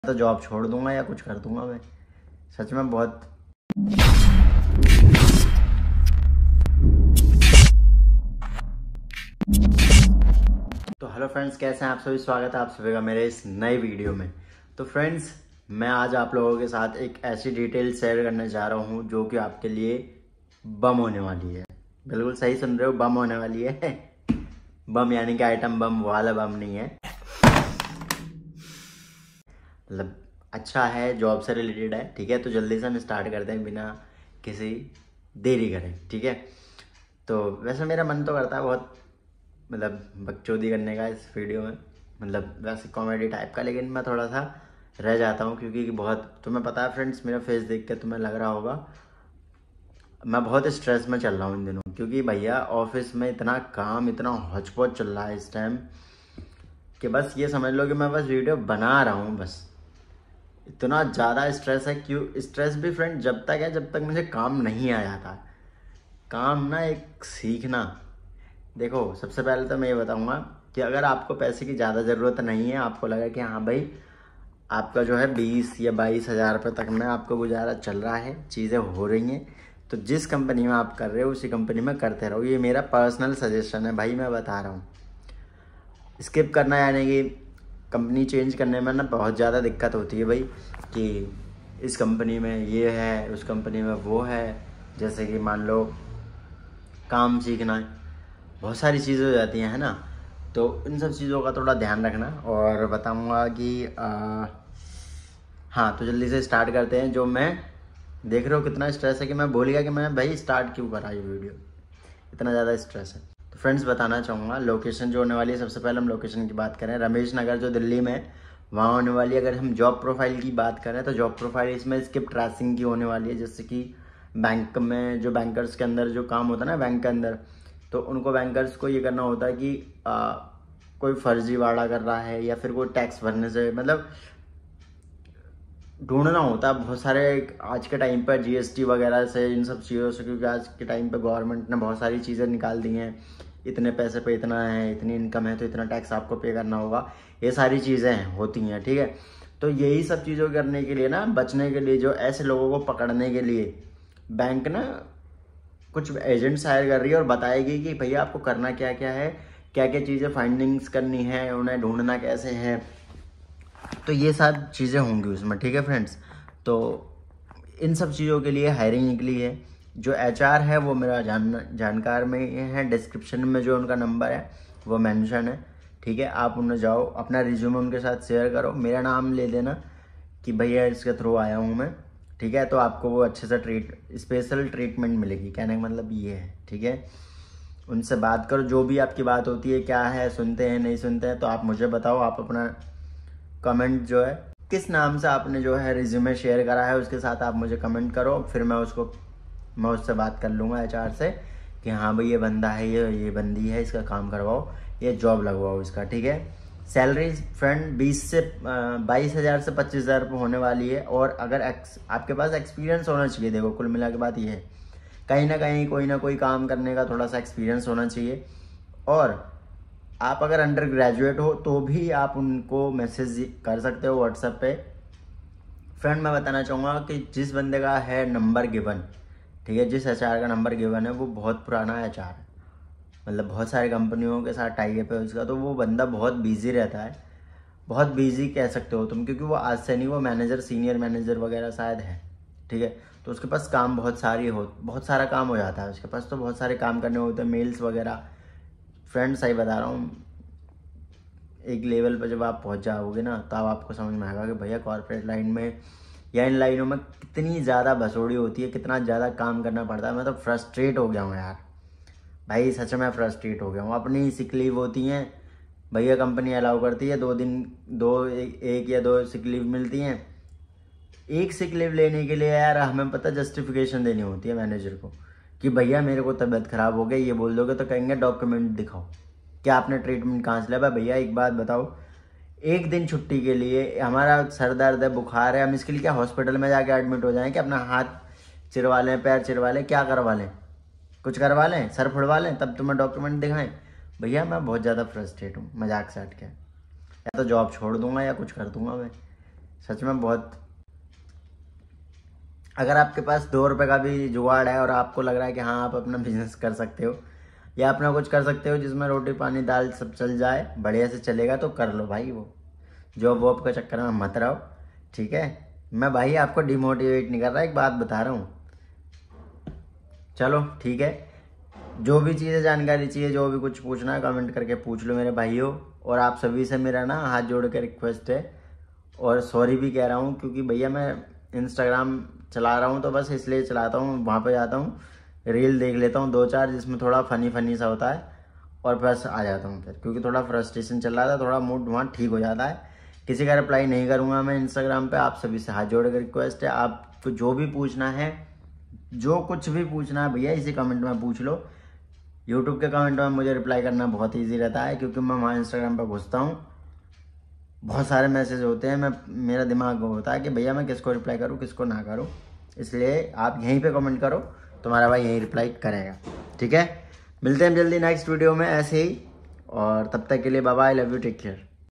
Job In Delhi | Bank Jobs In Delhi
We are Hiring for Skip Traceing
Location - Ramesh Nagar
Need Experience in Recovery and Collection
Salary 22k to 25 k
Contact Person
Rohit Joshi HR